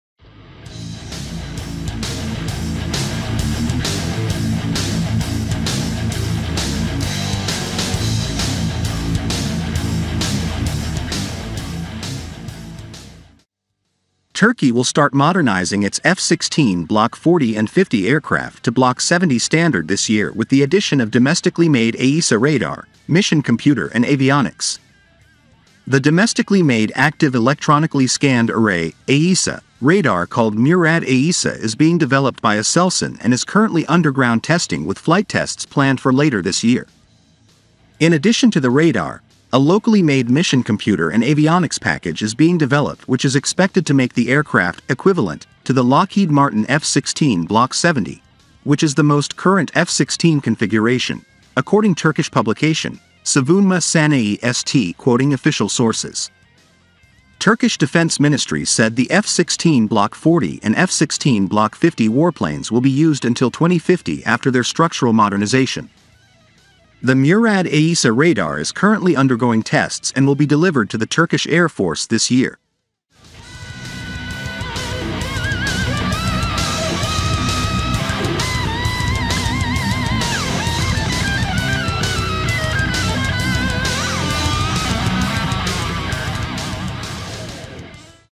Turkey to start Modernizing its F-16s to Block 70 Standard this Year
Turkey will start modernizing its F-16 Block 40 & 50 aircraft to Block 70 standard this year with the addition of domestically-made AESA Radar, mission computer and avionics.

The domestically-made active electronically scanned array (AESA) radar called MURAD AESA is being developed by ASELSAN and is currently underground testing with flight tests planned for later this year.